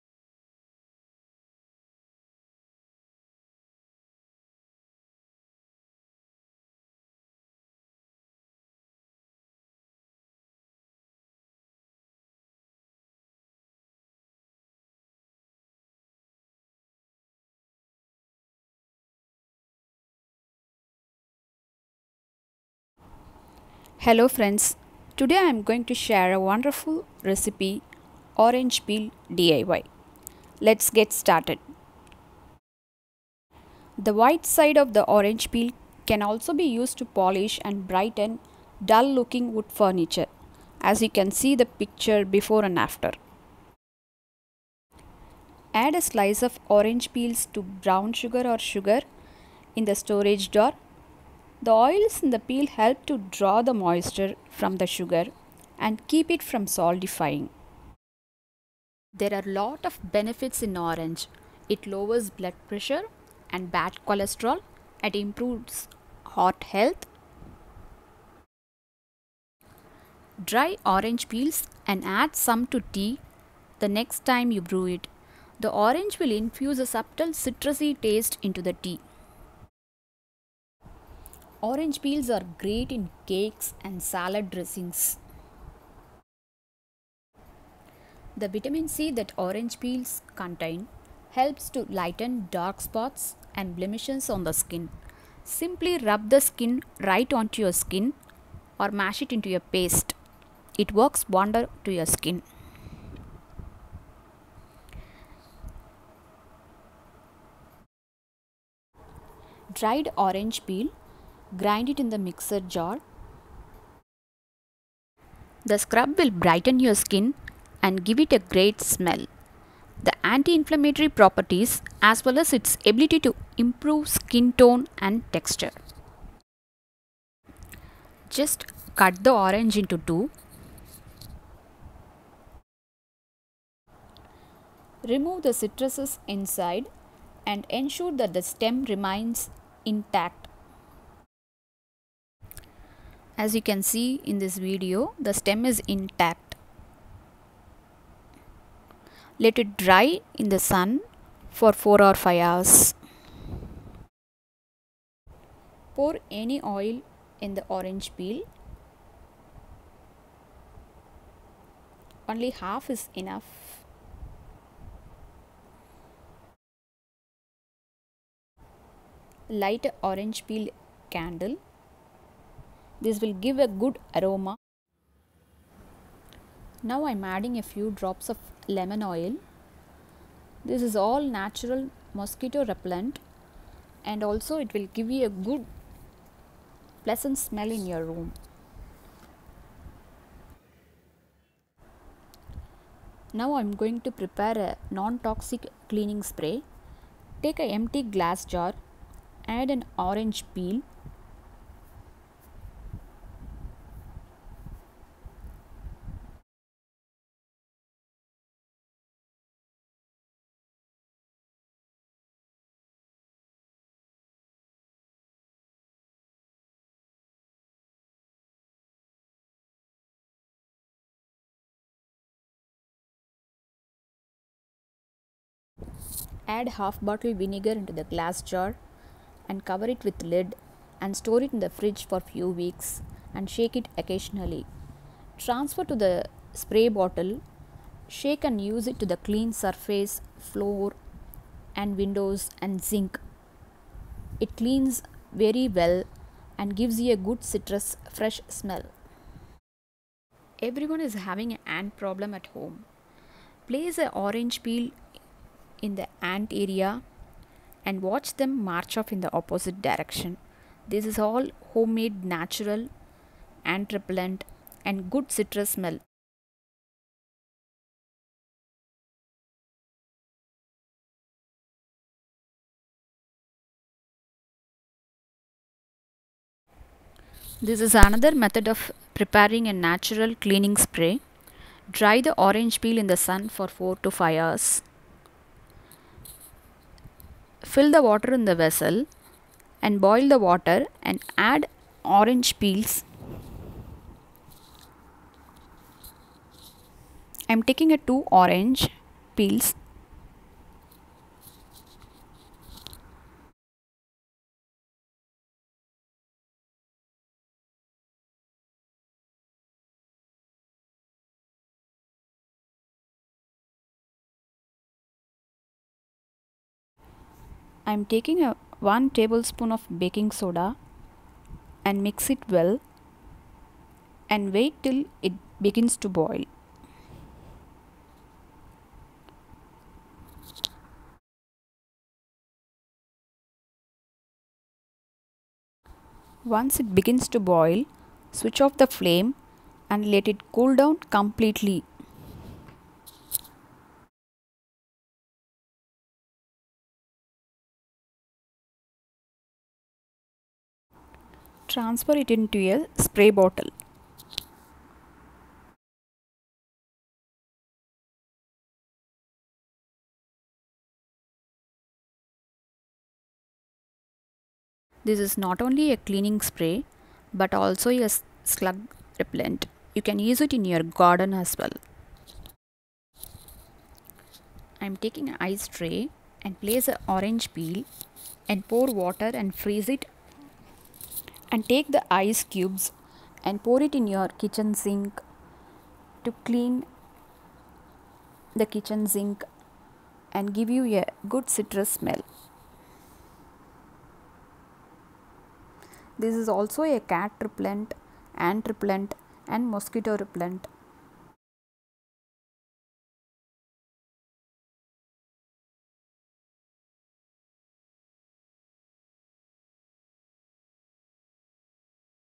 10 Amazing uses of Orange peel
There are loads of amazing ways to make the most wonderful recipe from the left over orange peel. 1. Taste Enhancer in cake & salad dressing. 2. Make a Tea with orange peel. 3. Natural cleansing spray. 4. Ant Repellent 5. Mosquito Repellent 6. Orange candle - Good Aroma 7. Slug Repellent in your garden 8. Body scrub / Face Scrub  9. stop brown sugar hardening 10. Polish wood.
Thanks